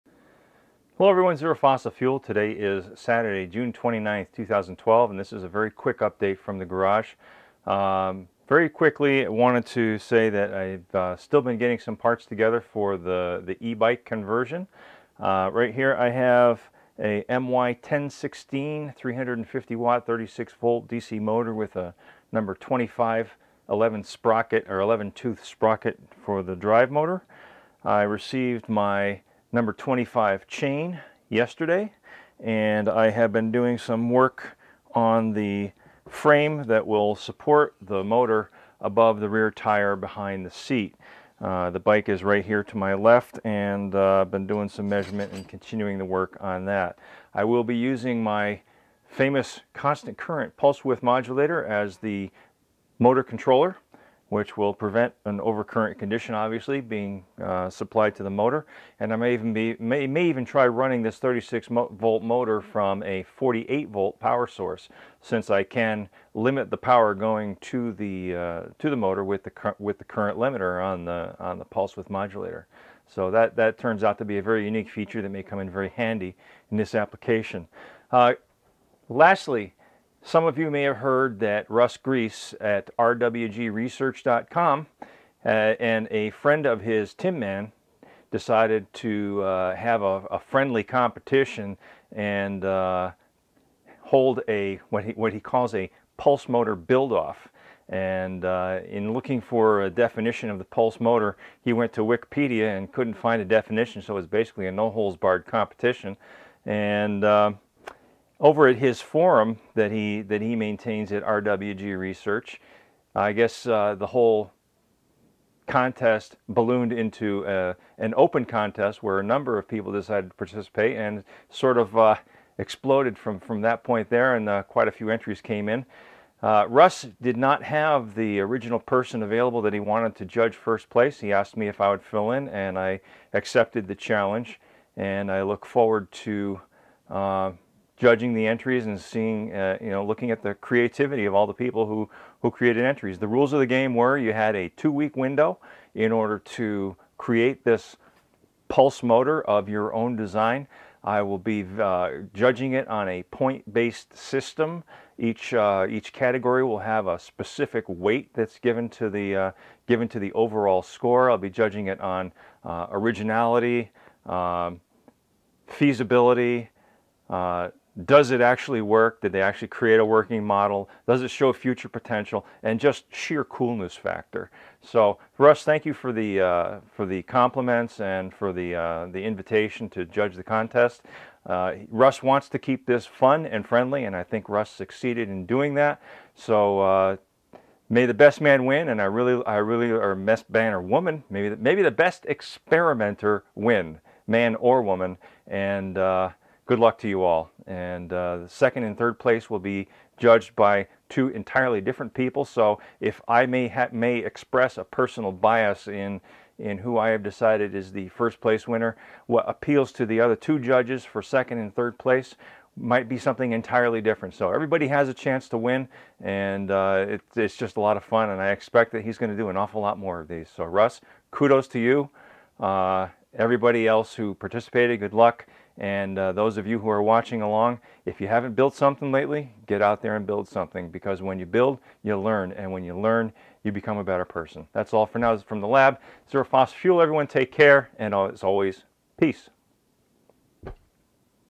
#309 RWG Pulse Motor build-off
RWG Research had a Pulse Motor Build-Off. Great prizes for 1st, 2nd & 3rd place. Winners get to choose. I'll be judging 1st place. Two other judges will decide 2nd & 3rd. Good luck to all! Link to the entries

Prizes donated by...
Kyle - Solar Panel
Sharky - VIC PCB
Peter - $$$ Gift cards

Also, parts are coming together nicely for the e-bike conversion.